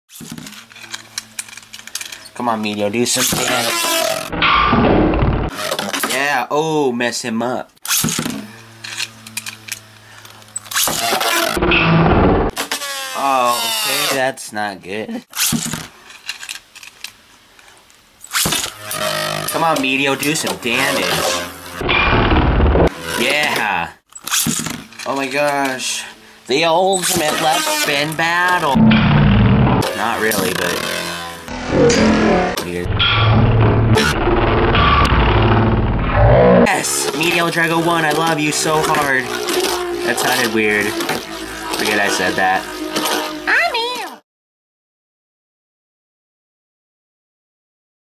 Gravity Perseus AD145WD vs Meteo L-Drago LW105LF!!!
An XTREME battle that we never got to see in the anime. Which bey is truly the strongest left rotating??!! LET IT RIP!!! :D